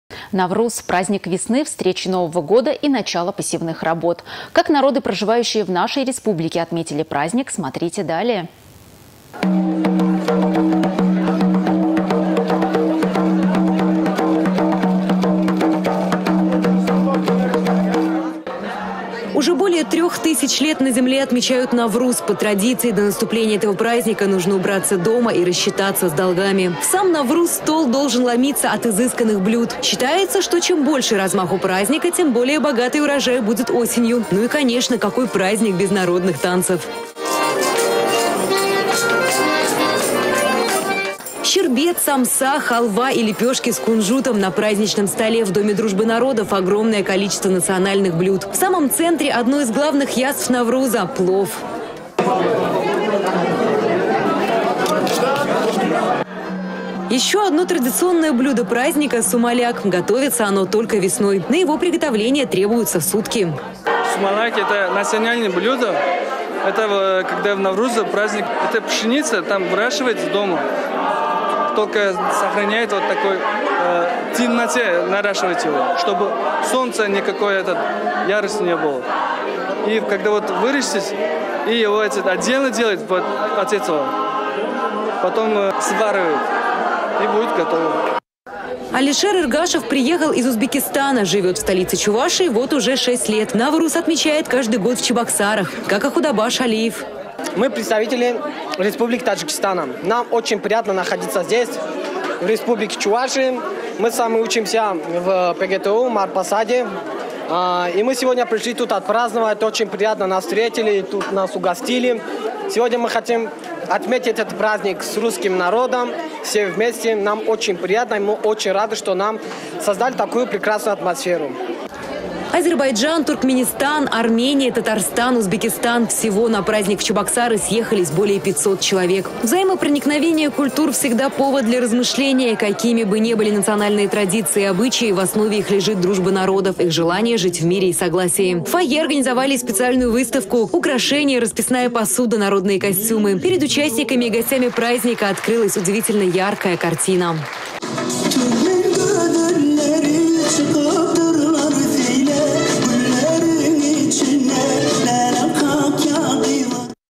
Навруз — праздник весны, встречи нового года и начала посевных работ.
Как  народы, проживающие в нашей республике,  отметили праздник, смотрите в нашем сюжете.